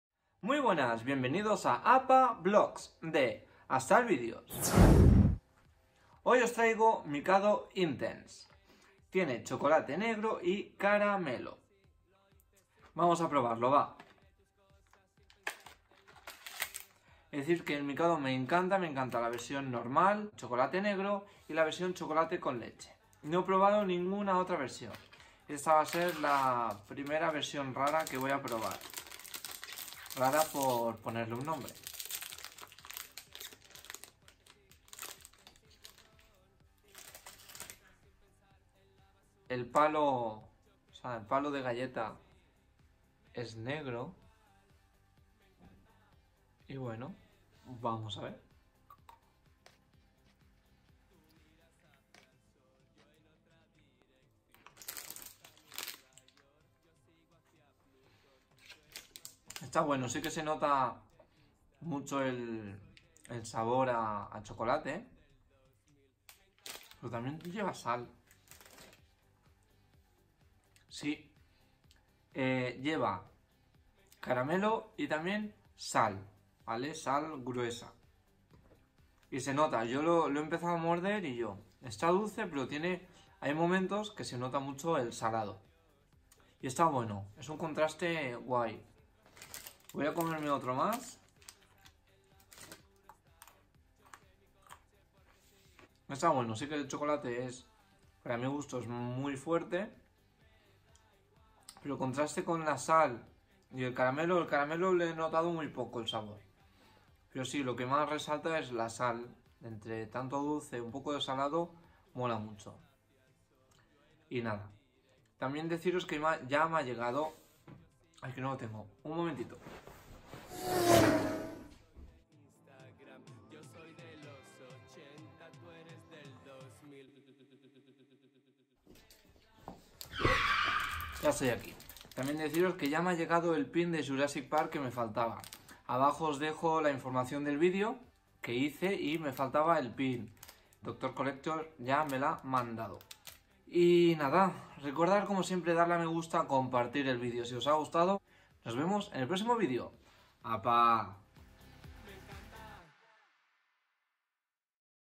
Probando Mikado Intense - APA Vlogs! | Astal Videos
¡Vuelvo a la carga a probar cosas de nuevo! Esta vez los nuevos Mikado Intense Caramelo con Sal y Chocolate Negro.